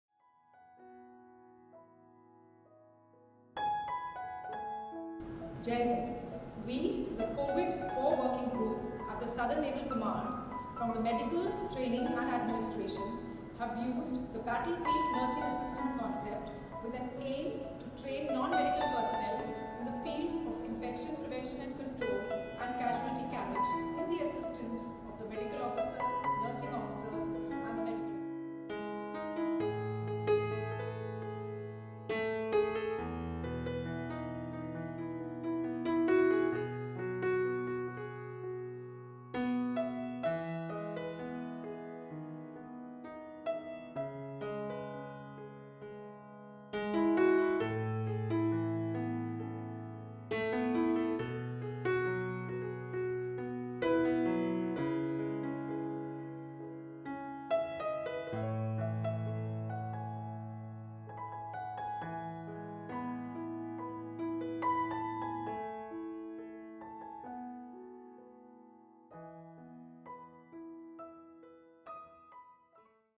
COVID-19: Indian Navy trains non-medical staff for emergency
Goa, Apr 05 (ANI): The COVID-29 core working group of Southern Naval Command has prepared a training capsule of Battle Field Nursing Assistant (BFNA) to train non-medical personnel who will work as force multipliers in times of emergencies. A total number of 333 personnel have been trained till date in Southern Naval Command.